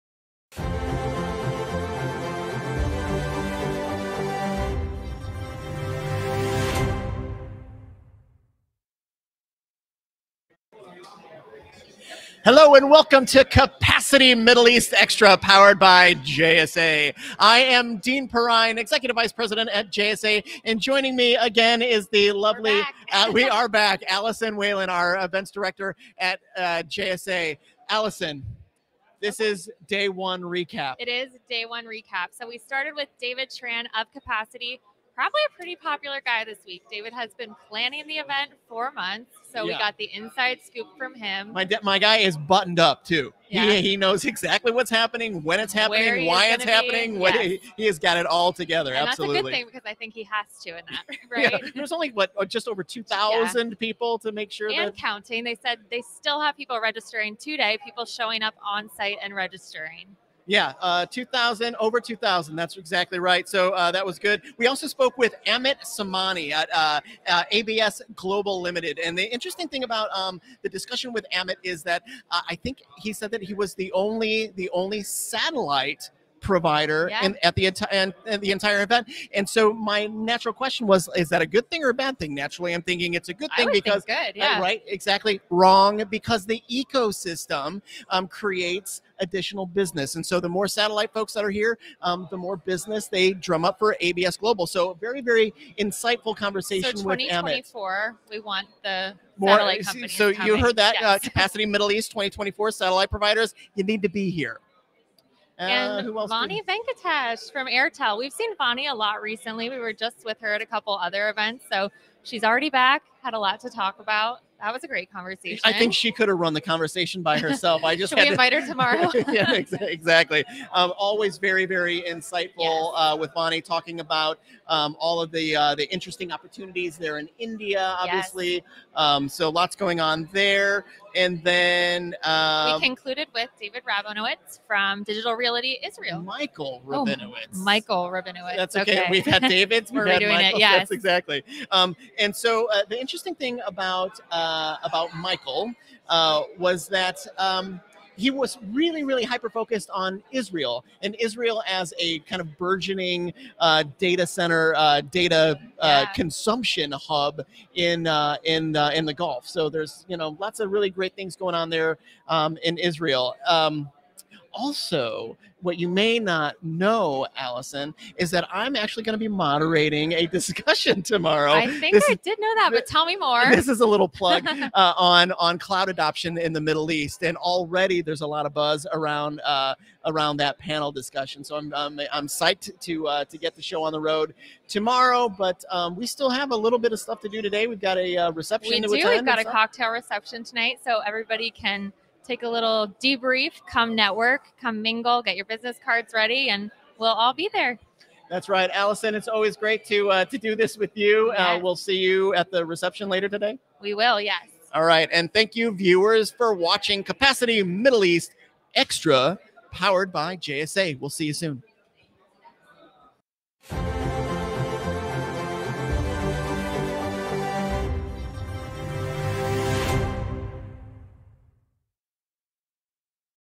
Capacity Middle East Xtra Powered by JSA | Day 1 Wrap-up with Allyson Whelan and Dean Perrine
Dean Perrine and Allyson Whelan broadcast LIVE from Dubai at Capacity Middle East. Together, Dean and Allyson recap day 1 of the event and share what to expect for day 2.